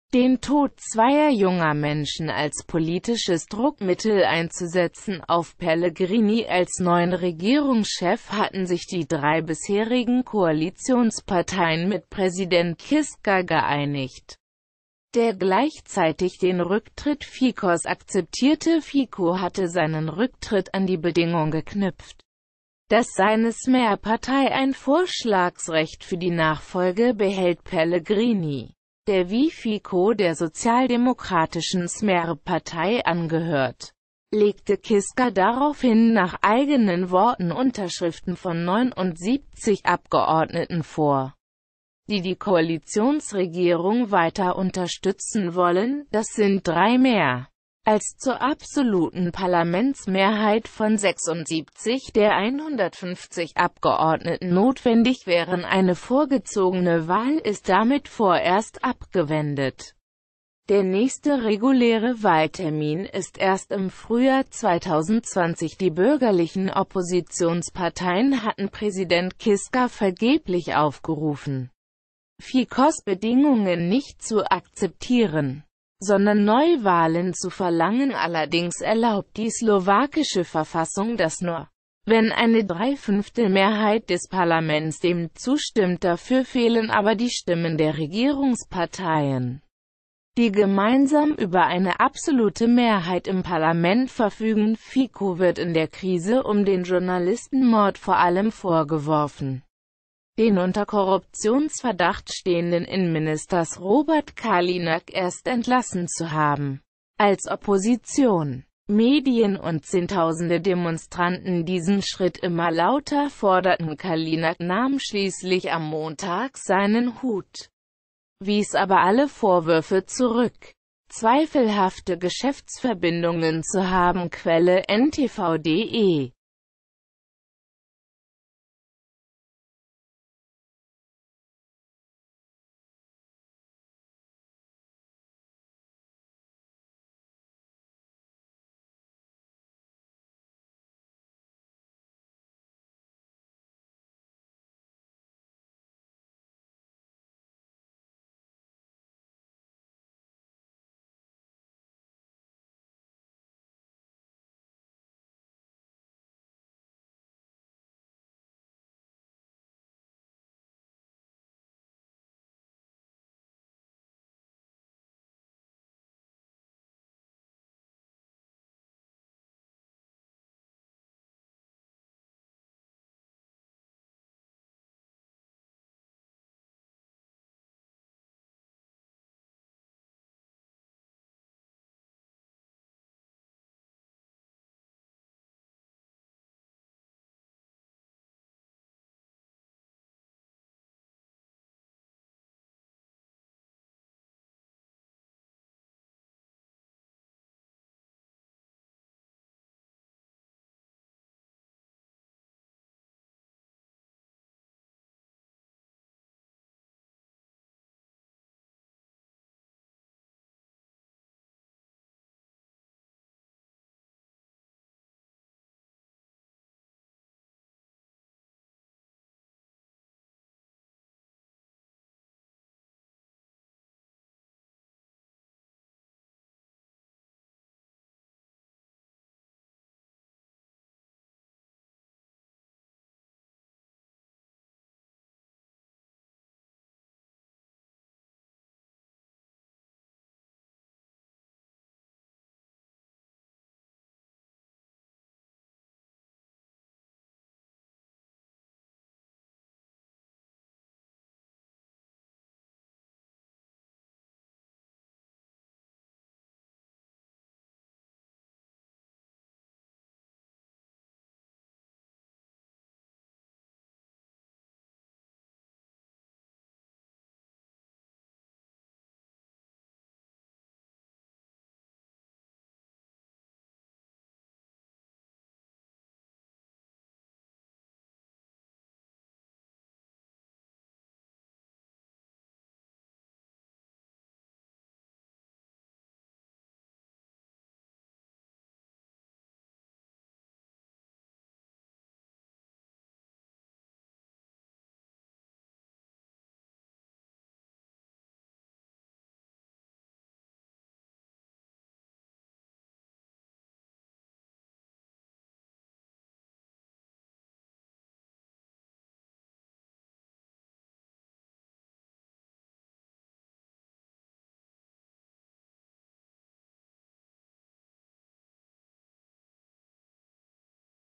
Keine Neuwahlen in der Slowakei: Pellegrini soll Regierungschef Fico ablösen
Thanks you verry much, like and sub video channel
。Der Mord an dem Enthüllungsjournalisten Kuciak stürzt die Slowakei in eine tiefe Krise Regierungschef Fico soll nun von seinem Vize abgelöst werden Neuwahlen sind damit erstmal vom Tisch - nicht aber die vielen Fragen nach den Mordhintergründen。Der slowakische Vizeregierungschef Peter Pellegrini soll Nachfolger von Regierungschef Robert Fico werden, der am Mittwochabend seinen Rücktritt angeboten hatte Das teilte Präsident Andrej Kiska in Bratislava mit Das Land steckt wegen der Ermordung des Enthüllungsjournalisten Jan Kuciak und dessen Freundin Martina Kusnirova vor drei Wochen in einer tiefen Krise。Der ermordete Journalist hatte in seinem letzten, erst nach seinem Tod veröffentlichten Artikel für das Nachrichtenportal aktualitysk über mutmaßliche Verbindungen zwischen der slowakischen Regierungspartei